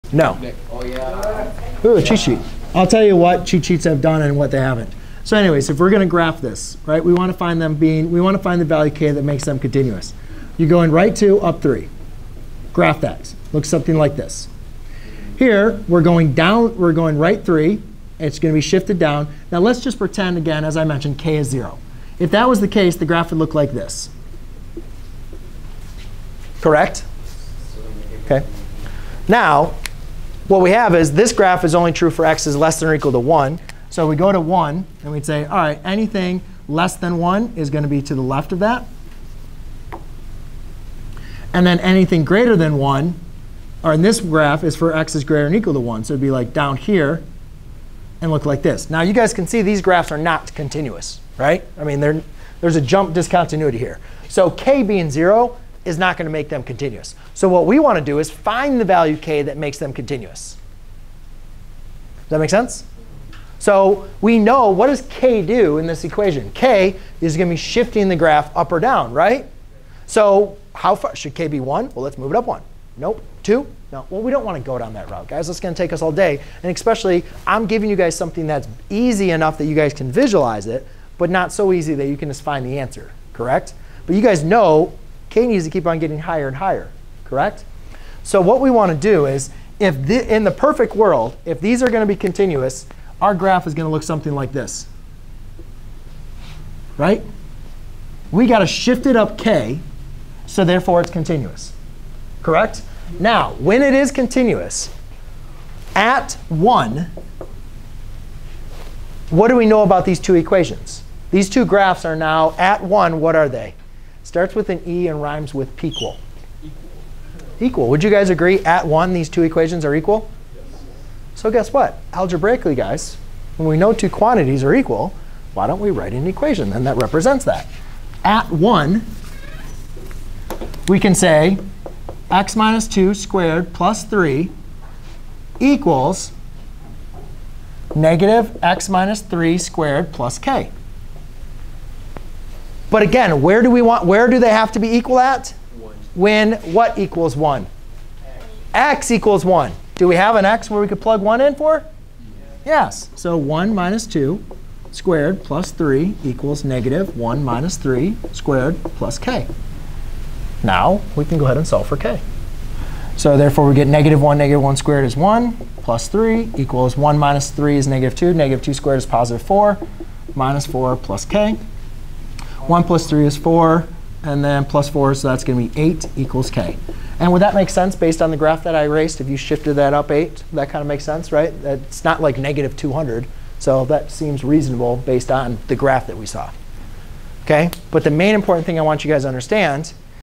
How to determine the value k that makes the function continuous
👉 Learn how to find the value that makes a function continuous. When given a piecewise function which has a hole at some point or at some interval, we fill the hole at the point or over the interval by looking for the value of the equation of the other constraints at the point or the interval where the hole is or we look for a different rule which gives us values that fills up the hole.